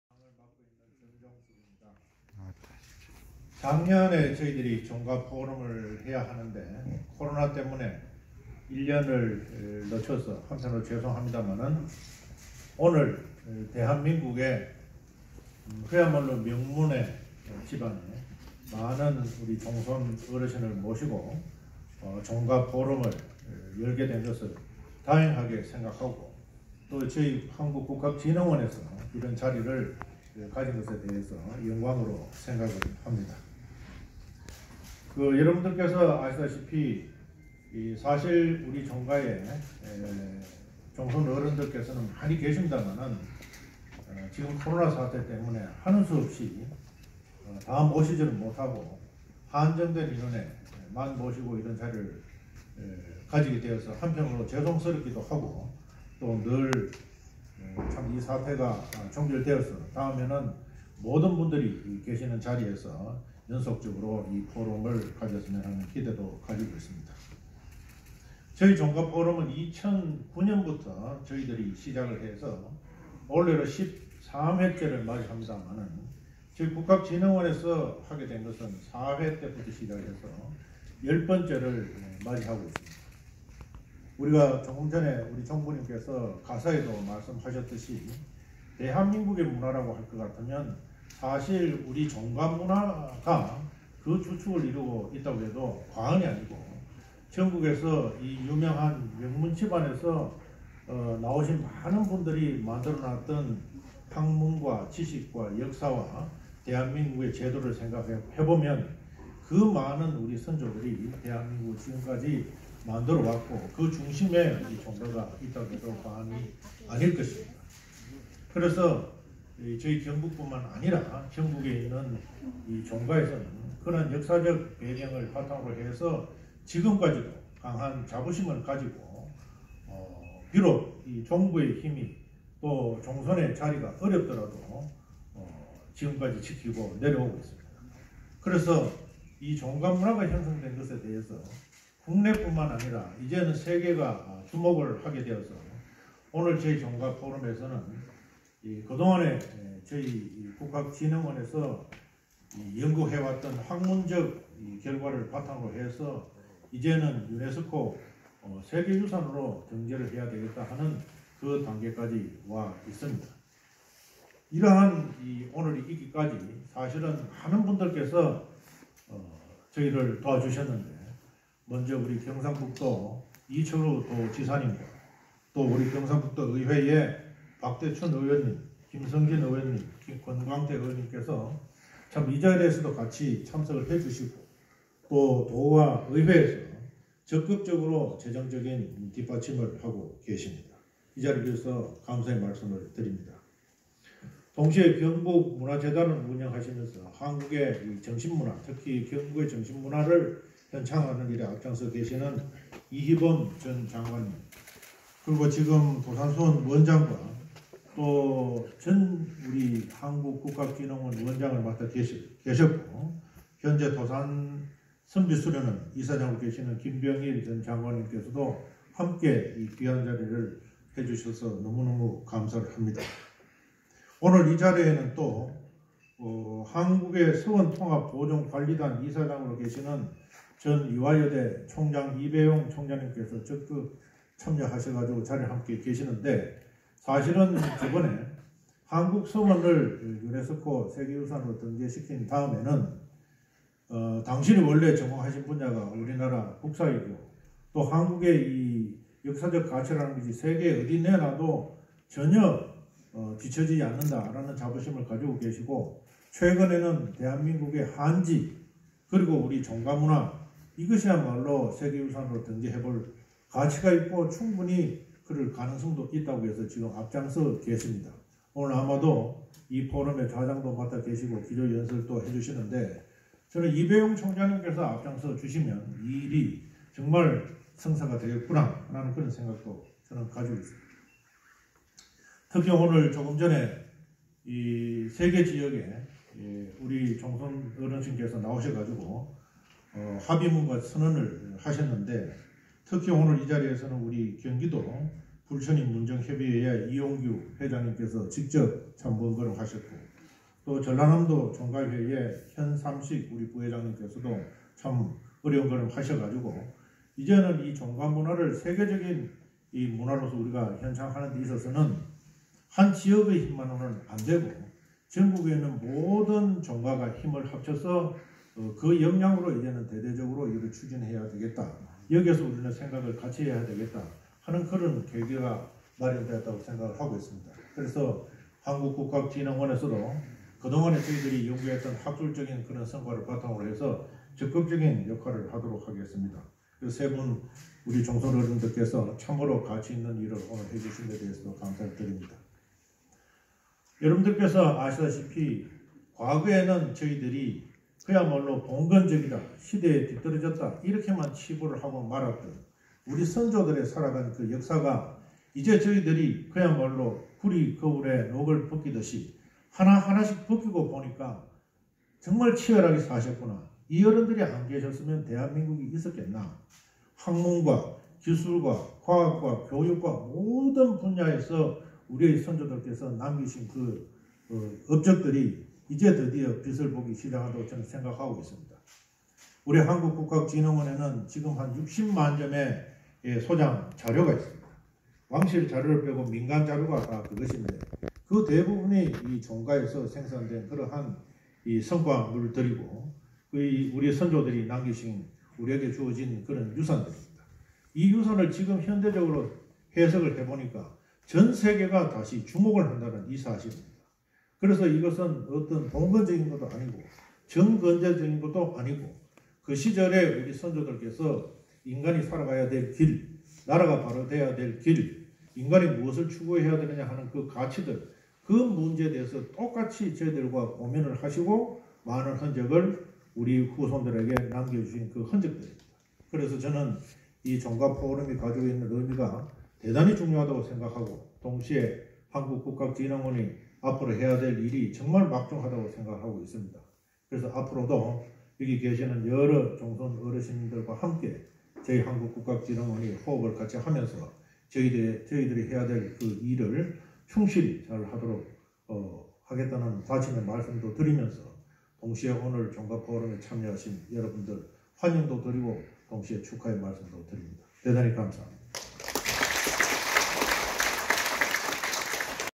제13회 종가포럼 개회사 ㅡ 한국 종가문화의 현황과 전망
21.10.19화
정종섭 한국국한진흥원장 개회사

제 13회 종가포럼 ㅡ 한국종가의 현황과 전망